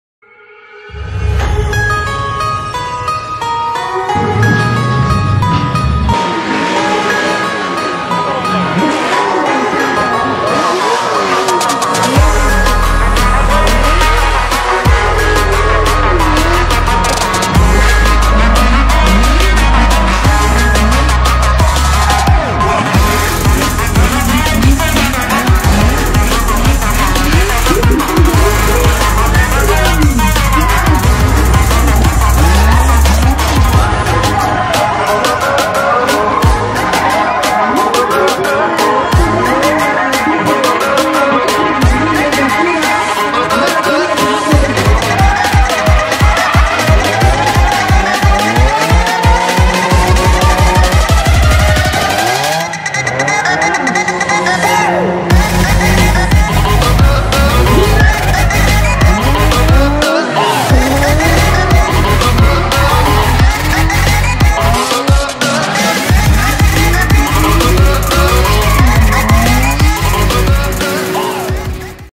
Red Bull Car Park Drift 2017 | Doha, Qatar
Beastly engines, and drivers roaring at the Dallah Parking in Corniche, Doha. This year's ranking:
1. ABDELRAHMAN FAKHROO 243 pts
2. AHMAD ABU IRSHAID 230 pts
3. MOHAMMAD AL KHAYAT 227 pts